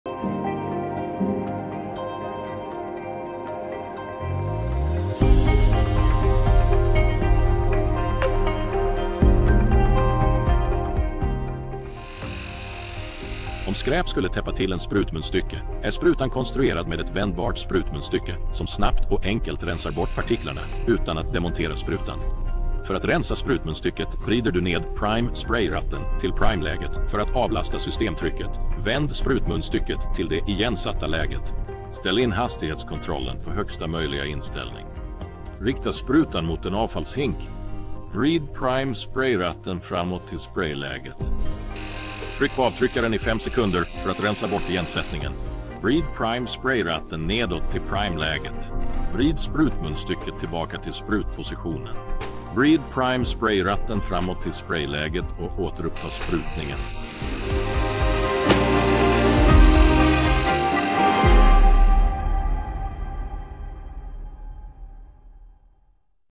Magnum by Graco TrueCoat 360 - Tutorial: Så här rensar du snabbt en igensatt sprutmunstycke
I händelse av att skräp blockerar sprutmunstycket
är sprutan utrustad med ett vändbart sprutmunstycke
som snabbt och enkelt rensar bort partiklarna utan att sprutan behöver tas isär.
Den här videon visar hur du rensar en igensatt sprutmunstycke från din TrueCoat 360-spruta.

► Kolla in vår INSTAGRAM eller FACEBOOK för mer innehåll!